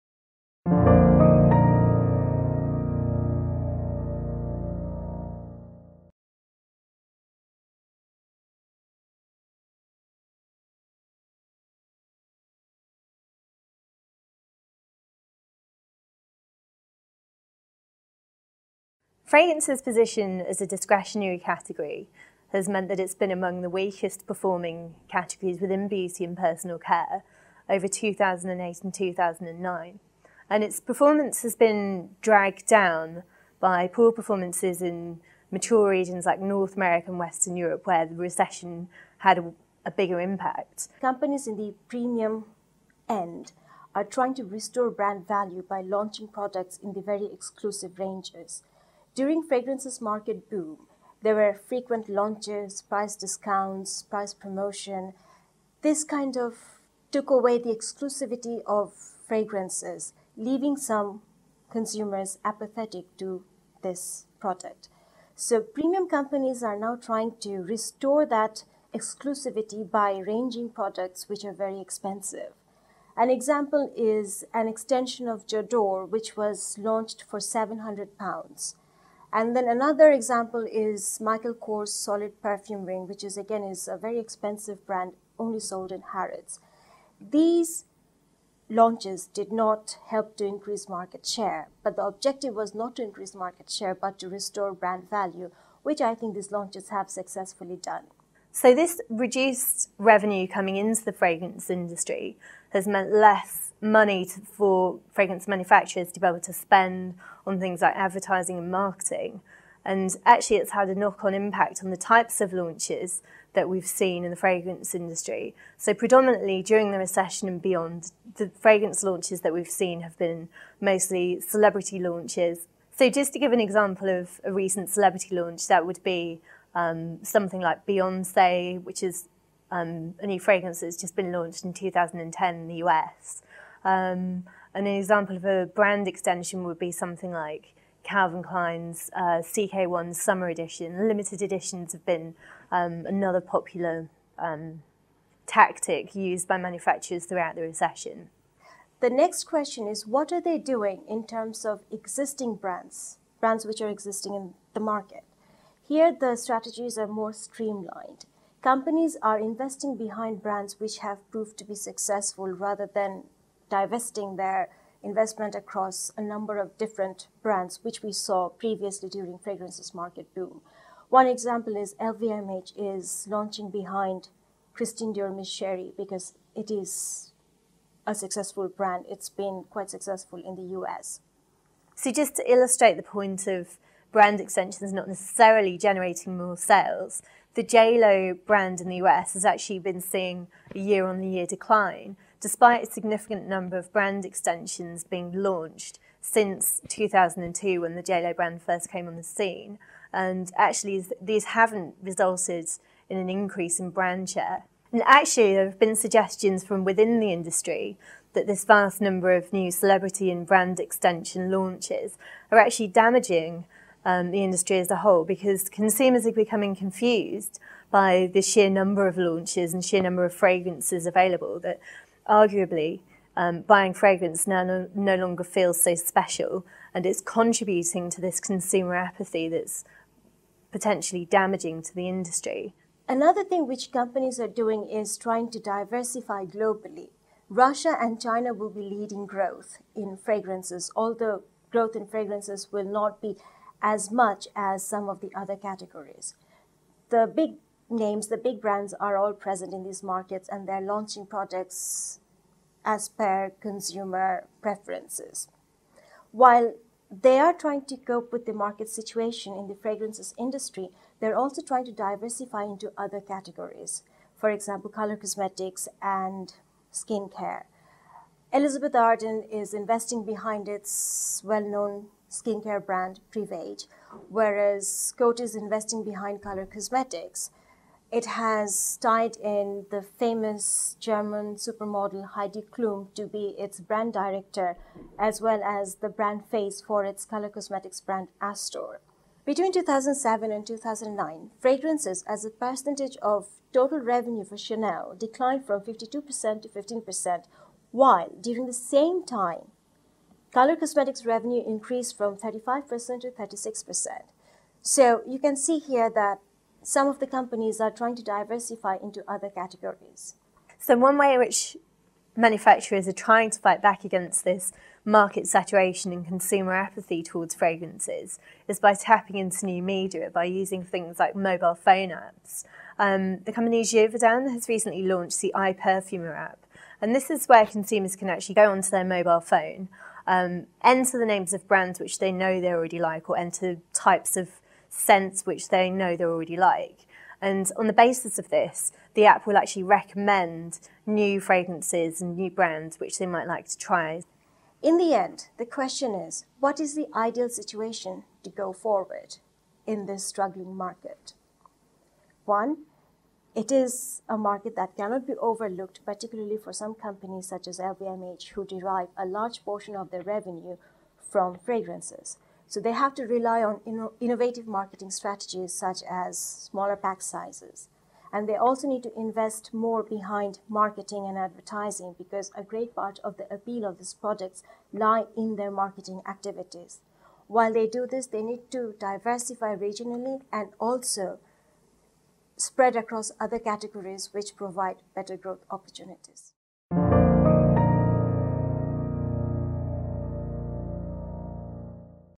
Corporate Strategies for Fragrances
Fragrances as a category of beauty and personal care has performed weakly over the past several years. According to Carrie Lenard, Beauty and Personal Care Industry Analyst at Euromonitor International, this poor performance has  dragged down in mature markets. Oru Mohiuddin, Beauty and Personal Care Industry Analyst, states that fragrance manufacturers are trying to restore brand value to products by launching exclusive products. Brands attempting this are:

J'adore
Michael Kors
Since the recession, revenue for the fragrance industry is down, which means less money to spend on marketing. This impacted what types of fragrances were recently launched - many were  celebrity launches or limited editions.

Companies have recently been investing in successful brands as well rather than marketing new brands. An example of this tactic is LVMH and the Chérie L'eau brand. Brand extensions and new brands have not been as popular - an example of this is the J'lo brand by Coty and it's extensions, which have declined  year on year.

Some industry insiders are blaming slow sales on the sheer number of celebrity launches and brand extensions, which they believe are over saturating the market and taking the luxury out of buying a fragrance.

Globalization of fragrances is another industry trend. The fastest growing markets for fragrances are:

Russia
China
Fragrance companies are also trying to diversify their product ranges, delving into color cosmetics and skin care. One of the more recent strategies used by the industry is tapping into technology by using mobile phone applications.

Mohiuddin concludes by stating there are several strategies that must be implemented by fragrance manufacturers  going forward:

Innovative product launches
Invest in marketing and advertising
Regional diversity
Spread across other beauty and personal care categories